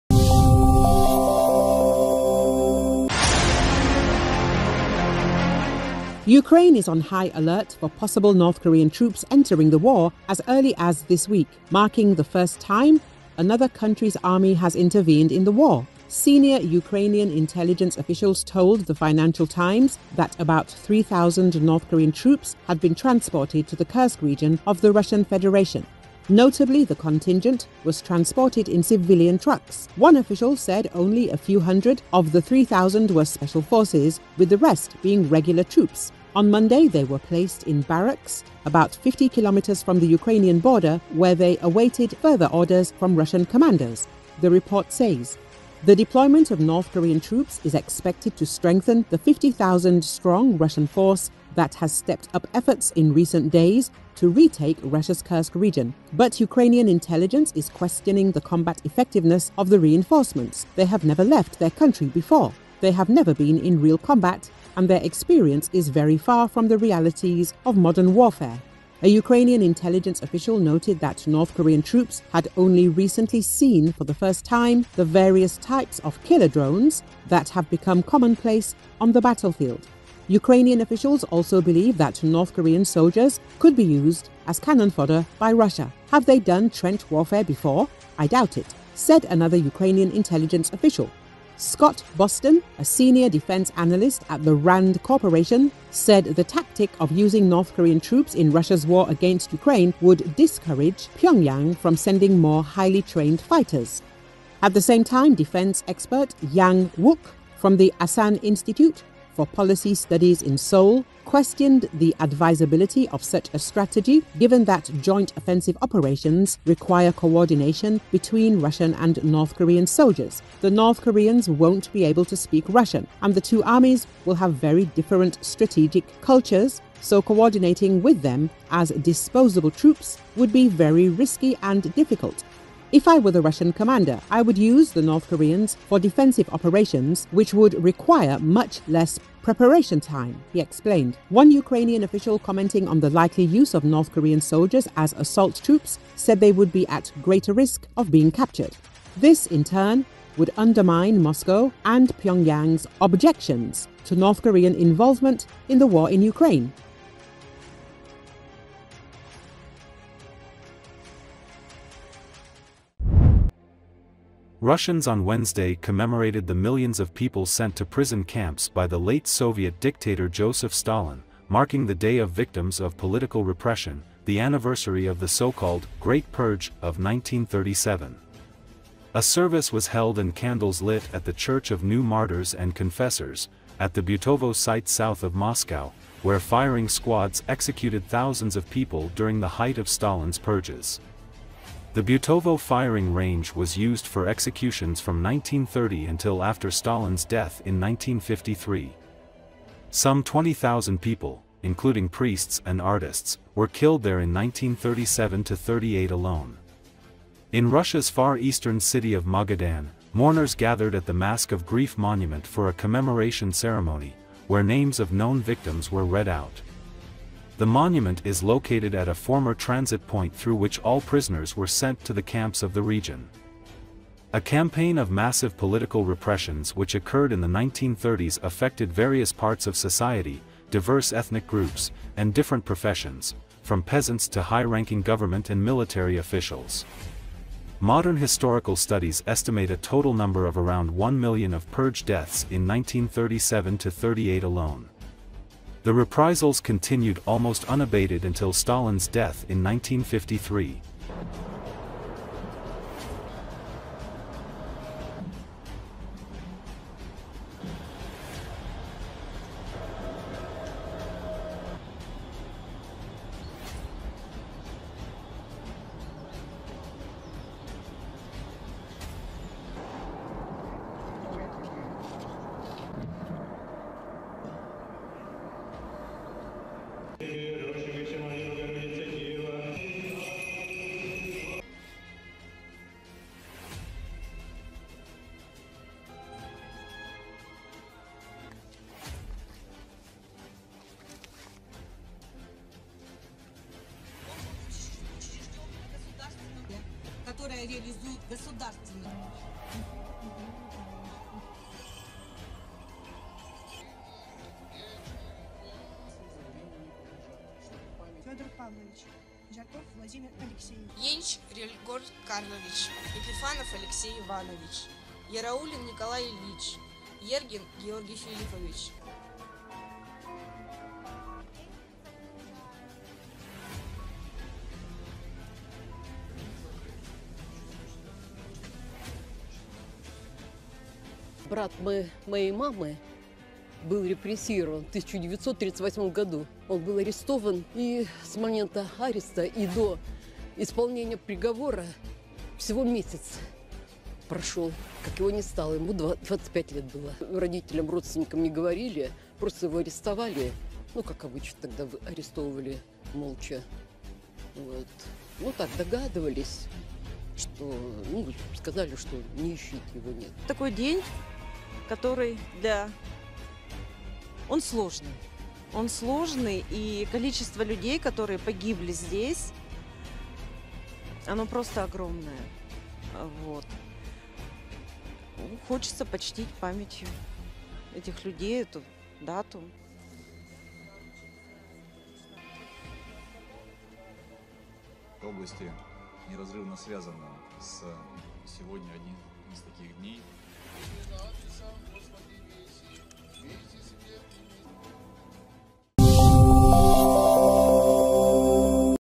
Many North Korean soldiers will be prisoners of Ukraine, they have never been in real combat
Ukraine is on high alert for possible North Korean troops entering the war as early as this week, marking the first time another country's army has intervened in the war.
Senior Ukrainian intelligence officials told the Financial Times that about 3,000 North Korean troops had been transported to the Kursk region of the Russian Federation. Notably, the contingent was transported in civilian trucks.
"One official said only a few hundred of the 3,000 were special forces, with the rest being regular troops. On Monday, they were placed in barracks about 50 kilometers from the Ukrainian border, where they awaited further orders from Russian commanders," the report says.
The deployment of North Korean troops is expected to strengthen the 50,000-strong Russian force that has stepped up efforts in recent days to retake Russia's Kursk region. But Ukrainian intelligence is questioning the combat effectiveness of the reinforcements:
"They have never left their country before... They have never been in real combat, and their experience is very far from the realities of modern warfare."
A Ukrainian intelligence official noted that North Korean troops had only recently seen for the first time the various types of killer drones that have become commonplace on the battlefield. Ukrainian officials also believe that North Korean soldiers could be used as "cannon fodder" by Russia.
"Have they done trench warfare before? I doubt it," said another Ukrainian intelligence official.
Scott Boston, a senior defense analyst at the Rand Corporation, said the tactic of using North Korean troops in Russia's war against Ukraine would discourage Pyongyang from sending more highly trained fighters.
At the same time, defense expert Yang Wook from the Asan Institute for Policy Studies in Seoul questioned the advisability of such a strategy, given that joint offensive operations require coordination between Russian and North Korean soldiers.
"The North Koreans won't be able to speak Russian, and the two armies will have very different strategic cultures, so coordinating with them as 'disposable troops' would be very risky and difficult. If I were the Russian commander, I would use the North Koreans for defensive operations, which would require much less preparation time," he explained.
One Ukrainian official, commenting on the likely use of North Korean soldiers as assault troops, said they would be at greater risk of being captured. This, in turn, would undermine Moscow and Pyongyang's objections to North Korean involvement in the war in Ukraine.